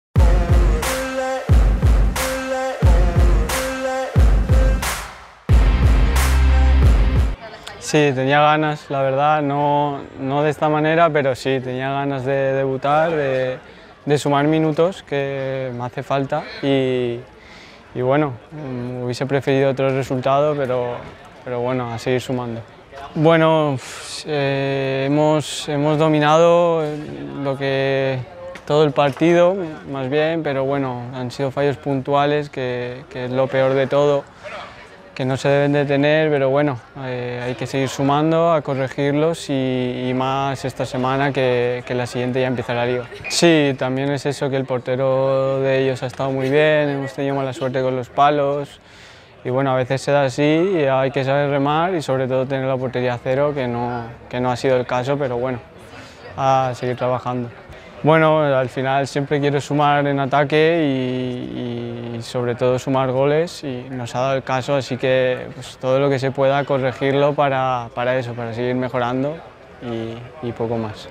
PRETEMPORADA 23-24 | Rueda de prensa Iván García| UD Almansa vs. Atzeneta UE
Declaraciones de Iván García tras debutar con la elástica 'taronja' en el octavo encuentro de pretemporada frente a la UD Almansa en el Estadio Municipal Paco Simón.